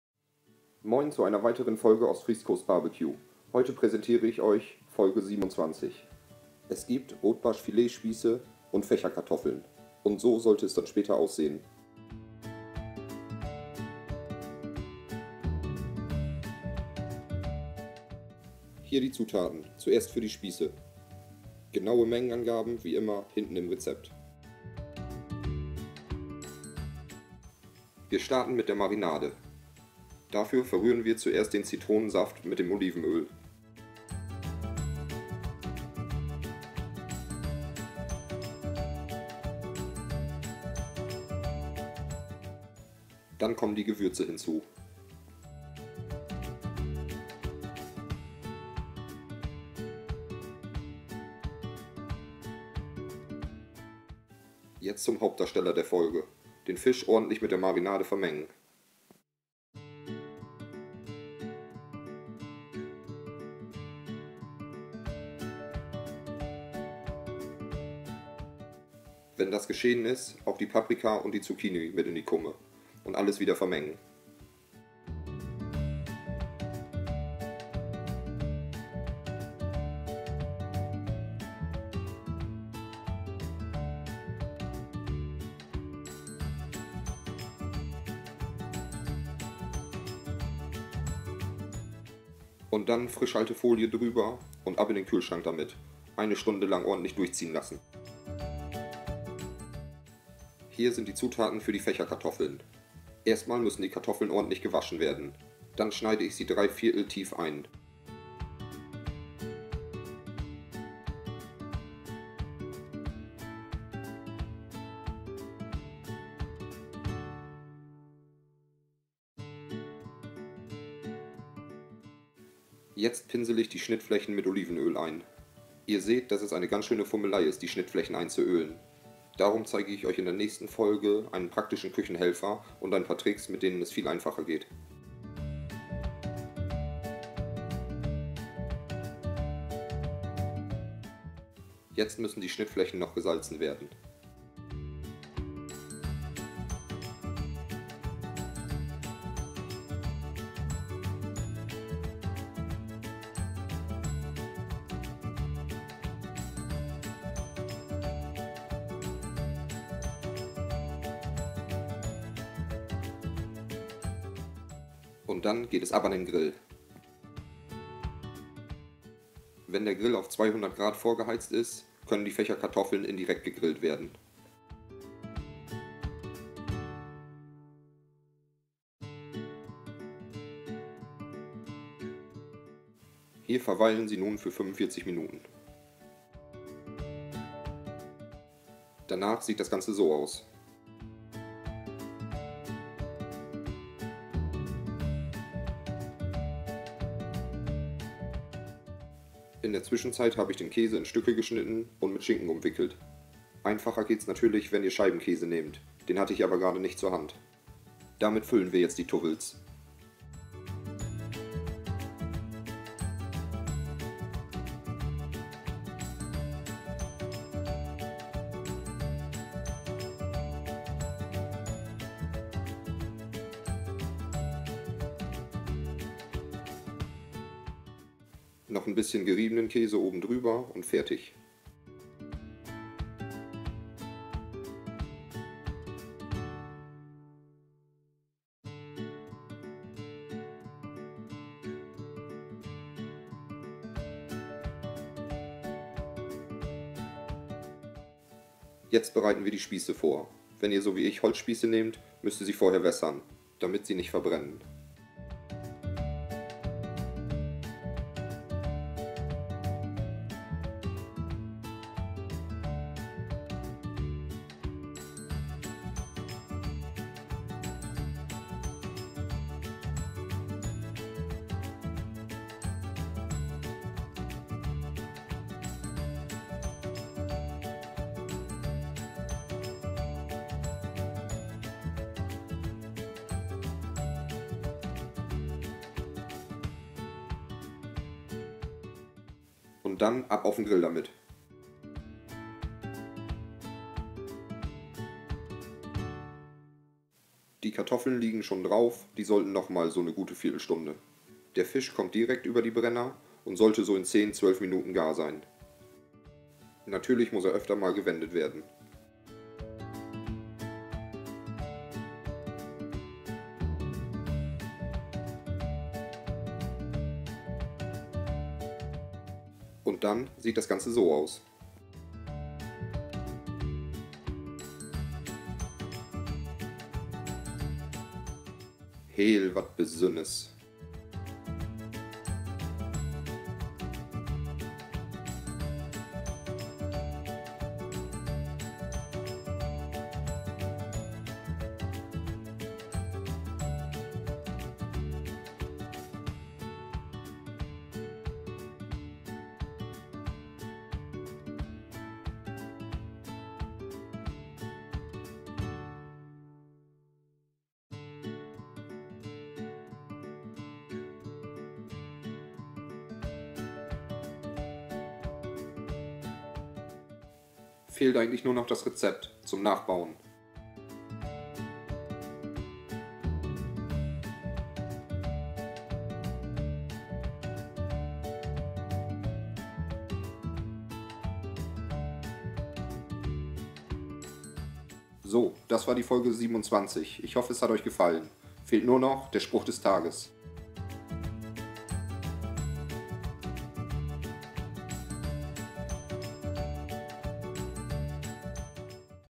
Rotbarschfilet-Spieße mit Fächerkartoffeln vom Gas-Grill / Der Grill-Friese #27
OSTFRIESCOAST BBQ!!!
Fisch vom Feinsten...